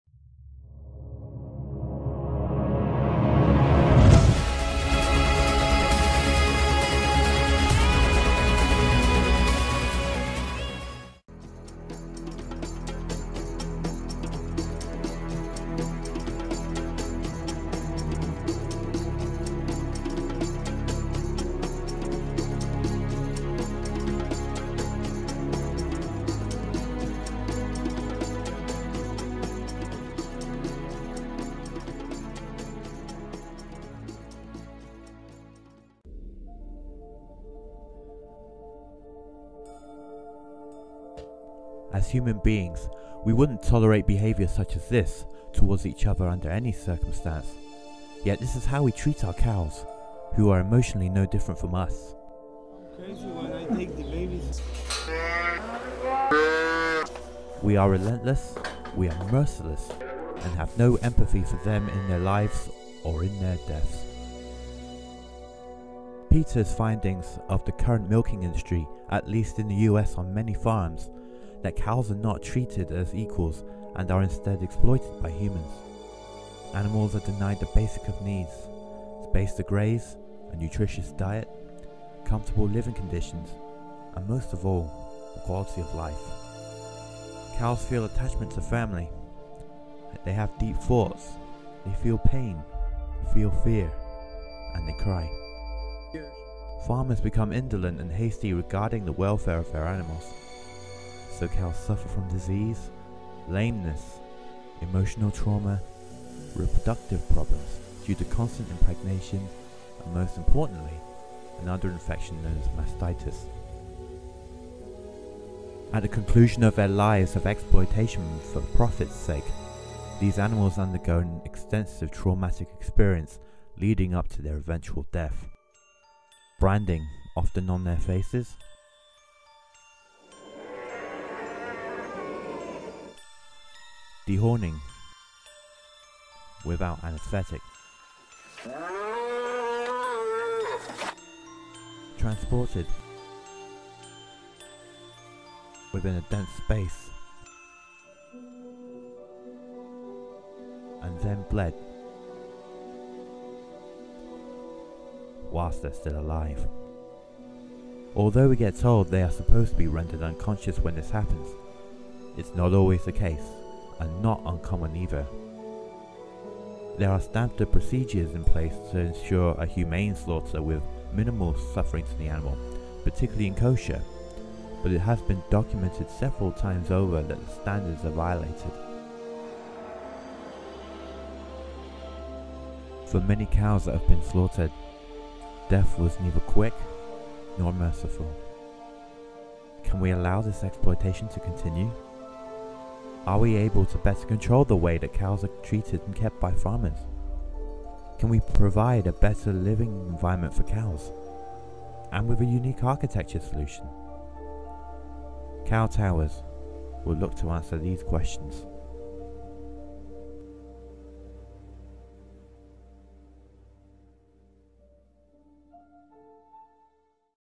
Cow Towers Part 1 - Cows' Abuse by Humans
Cow Towers is a revolutionary dairy farming facility that also incorporates sustainability and technology to harvest waste products from cows as well as the primary product of milk. Along with fulfilling future demand for milk in an ever growing world population, the facility will also fulfill the demand for renewable energy that would come from the recycling of dung and methane that cows create as a by product.

Part 1 outlines one environmental issue that the Cow Towers project looks to address. The video is based on initial research of cows' abuse by humans, their poor living conditions and the effects it has on them.

Music and certain footage used by owners described in acknowledgements and not by myself.